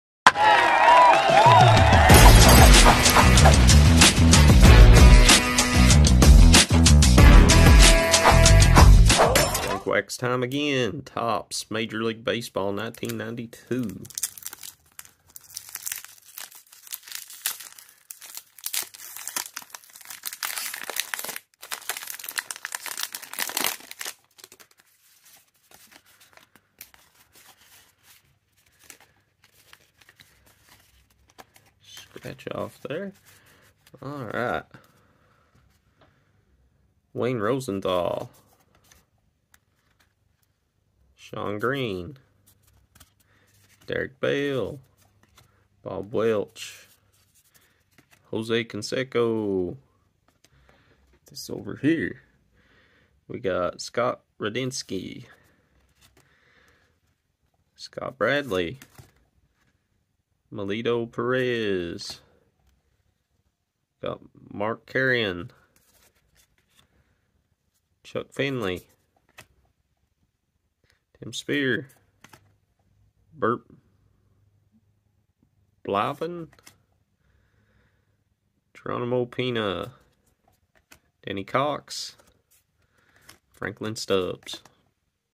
Junk wax time! Topps 1992
Ripping a New old stock pack of Topps baseball 1992 !